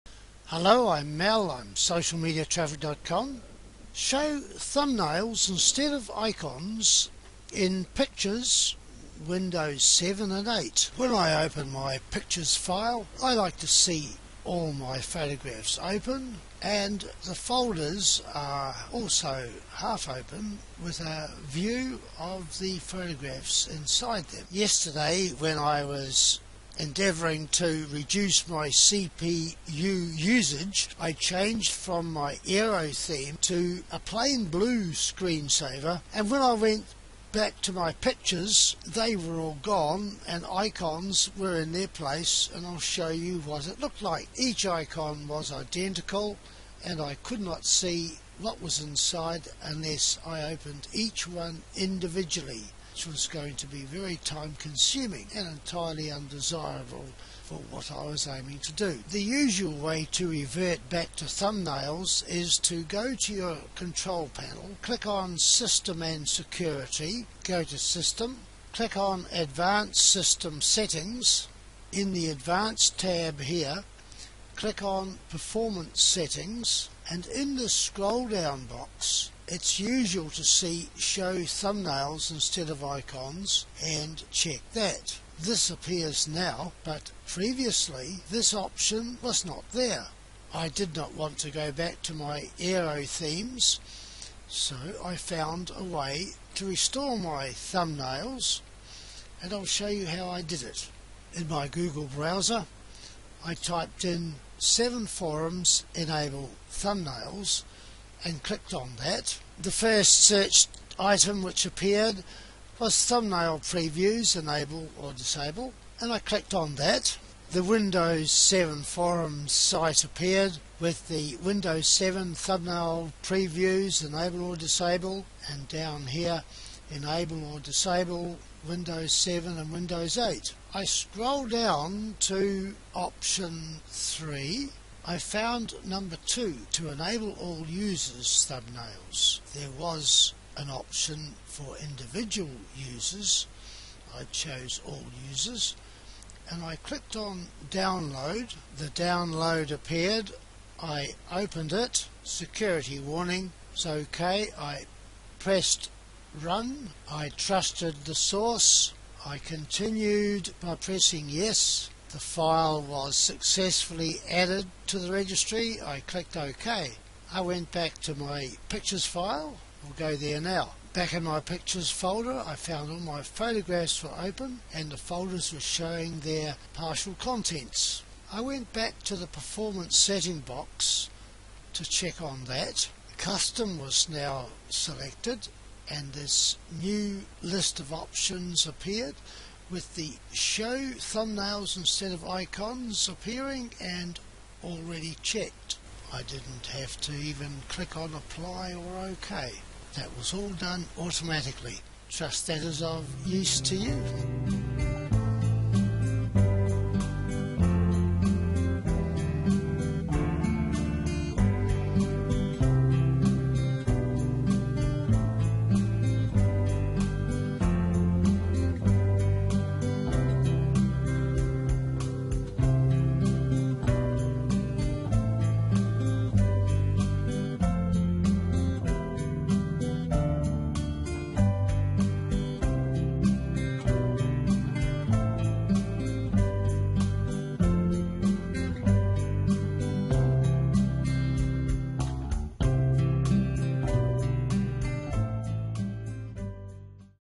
Show Thumbnails instead of Icons in Pictures Files Win7 & 8
WhenI open my Pictures file, I like to see all my Photos open & the folders are also open with a view of the Photos inside. Yesterday when I was endeavouring to reduce my CPU useage, I changed from my Aeros Themes to a plain blue screen saver. When I went back to my Pictures, they had all gone, Icons were in their place & I’ll show you what they looked like. Each Icon was identical & I couldn’t see what was inside unless I opened each one individually which was going to very time consuming & entirely undesirable for what I was aiming to do.
The usual way to revert back to thumbnails is to go to Control Panel, click Systems & Security, go to Systems, click on Advanced system settings. In the Advanced tab here, click on Performance settings & in this scroll down box, it’s usual to see “show thumbnails instead of Icons & check that. This appears now but previously this option was not there. I didn’t want to go back to my Aero Theme so I found another way to restore my Thumbnails & I’ll show you how I did it. In my browser I typed in my Google browser, I typed in 7 forums enable Thumbnails & clicked on that. The 1st search item which appeared was Thumbnail Previews, enable or disable & I clicked on that. The Windows 7 forum site appeared with Windows 7 Thumbnails Previews enable or disable & down here How to enable or disable, Win7 & 8. I scrolled down to Option 3. I found number 2 – to enable all users thumbnails. There was an opton for individual users, I chose all users. I clicked on Download, I opened it, Security warning, it’s Ok, I pressed Run, I trusted the source, I continued by pressing yes, the file was successfully added to the Registry. I went back to my Pictures file, I’ll go there now. I’m at my Pictures folder, I found all my Pictures were open & all the folders were showing their partial contents. I went back to the Performance settings box to check on that. custom was now selected, this new list of options appeared with the “Show thumbnails instead of Icons appearing & already checked. I didn’t even have to click on apply or Ok, it was all done automatically – trust that is of use to you.